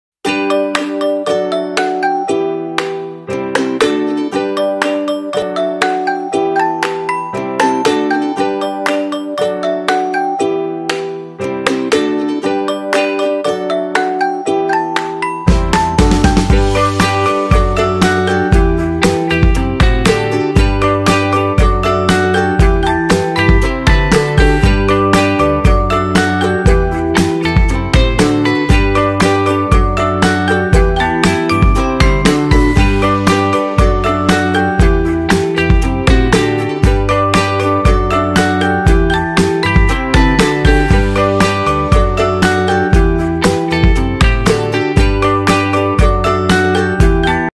hand craft / wedding gift /anniversary gift / sbse acha gift idea/ kisi ko shaadi gift kya de
If you also want to give a wedding gift to someone, then you will definitely give this hand crafted gift, it is very good and people are more attracted to it, you can also order me to make it and if you like this video If you like then do like and subscribe and definitely share my channel Do
mr.incredible_av
face book account atul vishwakarma